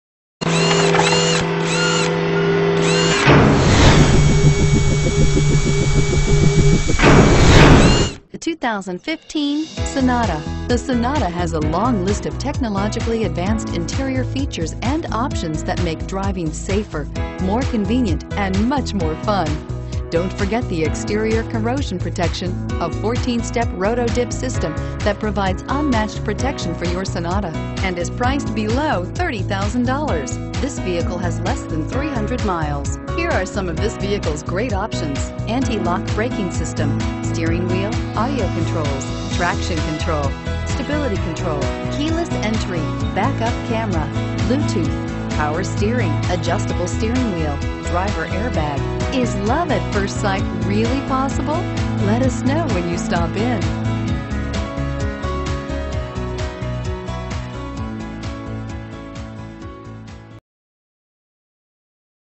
2015 Hyundai Sonata Irving TX HFH192284
2015 Hyundai Sonata 2.4L Sport

Freeman Auto
1840 E Airport Fwy

Looking for the right 4dr Car? Today could be your lucky day. With 266 miles, this Venetian Red, 2015 Hyundai Sonata equipped with Automatic transmission could be yours. Request more information and set up a test drive right away.